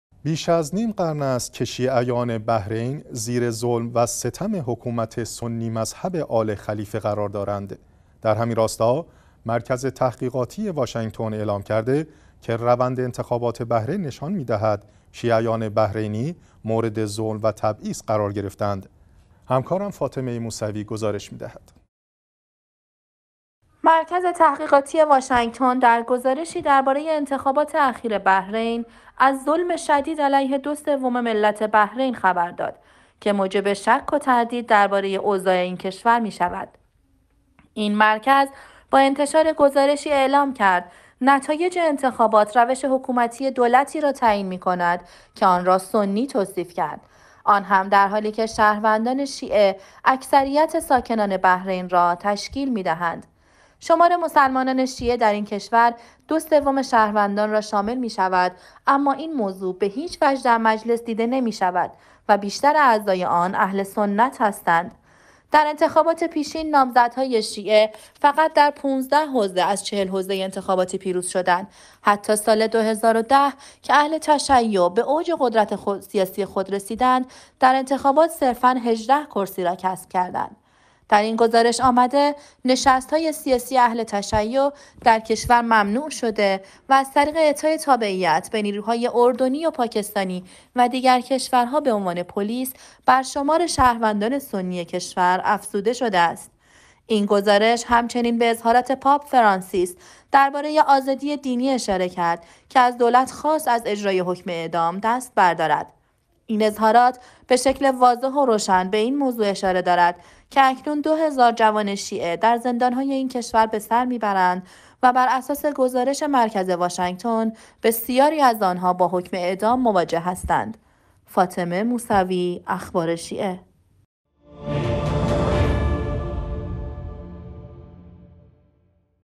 اذعان مرکز تحقیقاتی واشنگتن به ظلم و تبعیض علیه شیعیان بحرینی - فاطمه موسوی گزارش می دهد
بیش از نیم قرن است که شیعیان بحرین زیر ظلم و ستم حکومت سنی مذهب آل خلیفه قرار دارند.
 در همین راستا مرکز تحقیقاتی واشنگتن اعلام کرده که روند انتخابات بحرین نشان می‌دهد شیعیان بحرینی مورد ظلم و تبعیض قرار گرفته اند.

مرکز تحقیقاتی واشنگتن در گزارشی درباره انتخابات اخیر بحرین، از ظلم شدید علیه دو سوم ملت بحرین خبر داد که موجب شک و تردید درباره اوضاع این کشور می‌شود.
این مرکز با انتشار گزارشی اعلام کرد: نتایج انتخابات، روش حکومتی دولتی را تعیین می‌کند که آن را سنی توصیف کرد. آن هم در حالی که شهروندان شیعه اکثریت ساکنان بحرین را تشکیل می‌دهند.
شمار مسلمانان شیعه در این کشور دو سوم شهروندان را شامل می‌شود؛ اما این موضوع به هیچ وجه در مجلس دیده نمی‌شود و بیشتر اعضای آن اهل سنت هستند.
در انتخابات پیشین، نامزد‌های شیعه فقط در ۱۵ حوزه از ۴۰ حوزه انتخاباتی پیروز شدند. حتی سال ۲۰۱۰ که اهل تشیع به اوج قدرت سیاسی خود رسیدند، در انتخابات صرفا ۱۸ کرسی را کسب کردند.
در این گزارش آمده نشست‌های سیاسی اهل تشیع در کشور ممنوع شده و از طریق اعطای تابعیت به نیرو‌های اردنی و پاکستان و دیگر کشور‌ها در پلیس بر شمار شهروندان سنی کشور افزوده شده است.
این گزارش همچنین به اظهارات پاپ فرانسیس درباره آزادی دینی اشاره کرد که از دولت خواست از اجرای حکم اعدام دست بردارد.
این اظهارات به شکل واضح و روشن به این موضوع اشاره دارد که اکنون دو هزار جوان شیعه در زندان‌های این کشور به سرمی برند و براساس گزارش مرکز واشنگتن، بسیاری از آن‌ها با حکم اعدام مواجه هستند.